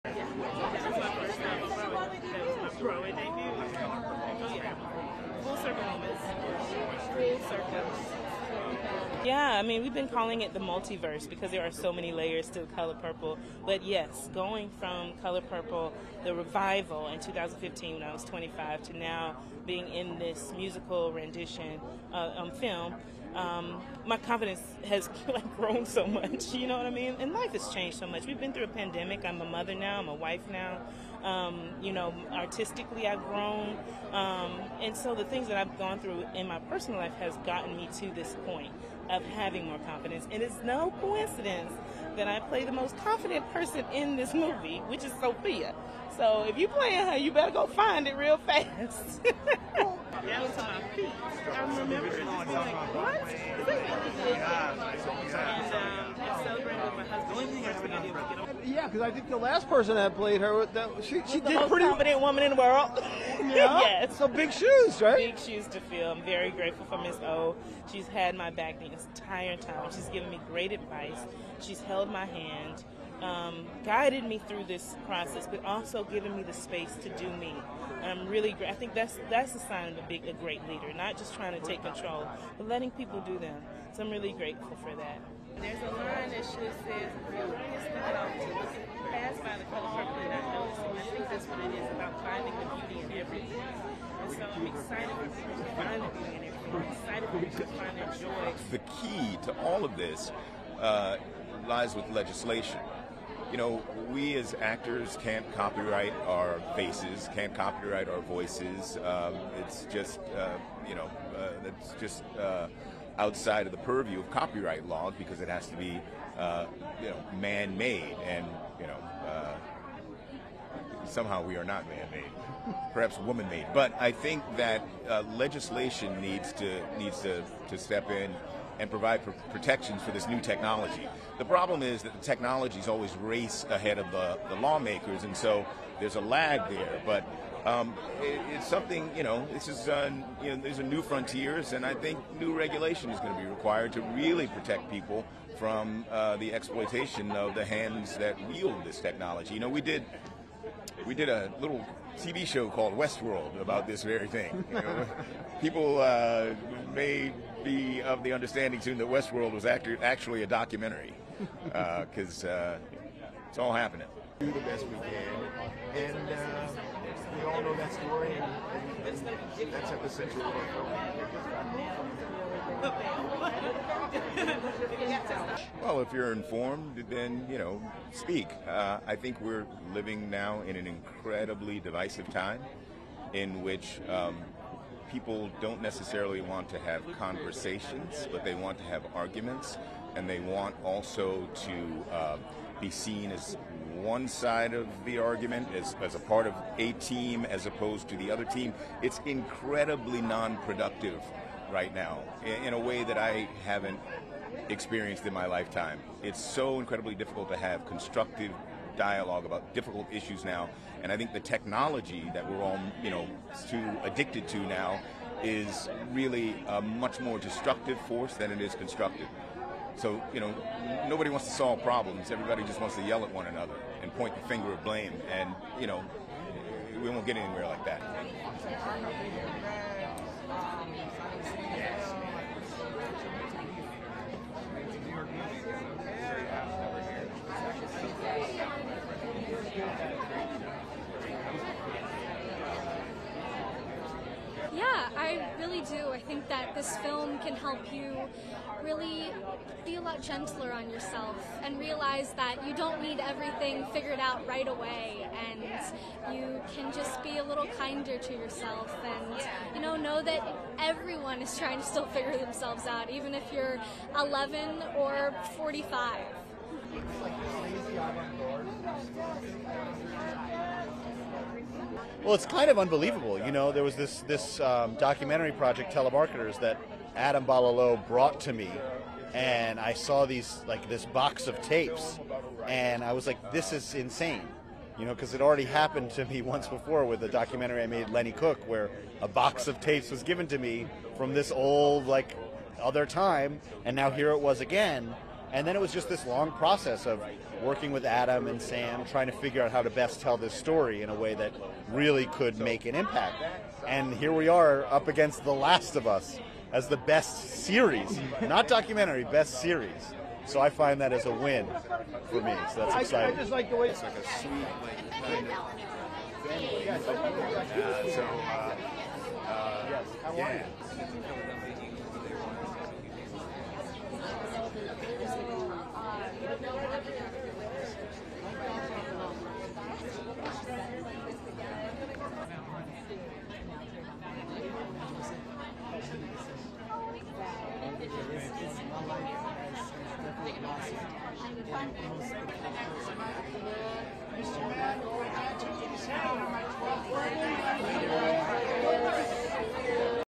Danielle Brooks, Jeffrey Wright, Benny Safdie arrive at Gotham Awards
ASSOCIATED PRESS
New York - 27 November 2023
1. Zoom out of Danielle Brooks talking with press
2. SOUNDBITE (English) Danielle Brooks, actor:
“We've been calling it the multiverse because there are so many layers to 'Color Purple.' But yes, going from 'Color Purple,' the revival in 2015, when I was 25 to now being in this musical rendition on film, my confidence has grown so much. You know what I mean? And life has changed so much. We've been through a pandemic. I'm a mother now. I'm a wife now. You know, artistically I've grown. And so the things that I've gone through in my personal life has gotten me to this point of having more confidence. And it's no coincidence that I play the most confident person in this movie. Which is Sofia? So if you play, you better go find it real it fast.”
3. Tight of Danielle Brooks talking with press
4. SOUNDBITE (English) Danielle Brooks, actor - On Oprah:
(Reporter: “Yeah, because I think the last person I played her with that she did pretty…”)
“Most confident woman in the world.”
(Reporter: “Yeah. It's a big shoes.”
“Big shoes to fill. I’m very grateful for Ms. O (Oprah). So she's had my back this entire time. She's giving me great advice. She's held my hand, guided me through this crisis, but also given me the space to do me. And really, I think that's that's a sign of being a great leader, not just trying to take control and letting people do that. So I'm really grateful for that.”
5. Zoom in of Danielle Brooks talking with press
6. SOUNDBITE (English) Jeffrey Wright, actor - On AI and its effect on actors and the entertainment industry :
“The key to all of this lies with legislation. You know, we as actors can't copyright our faces, can't copyright our voices. It's just, you know, that's just outside of the purview of copyright law because it has to be man made. And, you know. Somehow we are not man made, perhaps woman made. But I think that legislation needs to needs to step in and provide for protections for this new technology. The problem is that the technology has always race ahead of the lawmakers and so there's a lag there. But it is something you know, it's done and there's a new frontiers. And I think new regulation is going to be required to really protect people from the exploitation of the hands that wield this technology. You know, we did a little TV show called 'Westworld' about this very thing. People may be of the understanding that 'Westworld' was actually a documentary because it's all happening.”
7. Zoom in on Jeffrey Wright talking with press
“Well, if you're informed, then, you know. Speak. I think we're living now in an incredibly divisive time in which people don't necessarily want to have conversations, but they want to have arguments and they want also to be seen as one side of the argument is as a part of a team as opposed to the other team. It's incredibly nonproductive right now in a way that I haven't experienced in my lifetime. It's so incredibly difficult to have constructive dialogue about difficult issues now. And I think the technology that we're all, you know, too addicted to now is really a much more destructive force than it is constructive. So, you know, nobody wants to solve problems. Everybody just wants to yell at one another and point a finger of blame. And, you know, we won't get anywhere like that.”
9. Medium of Steven Yeun talking with press
10. SOUNDBITE (English) Abby Ryder Fortson, actor - on the influence of “Are You There God? It's Me, Margaret.”:
11. Tight of Benny Safdie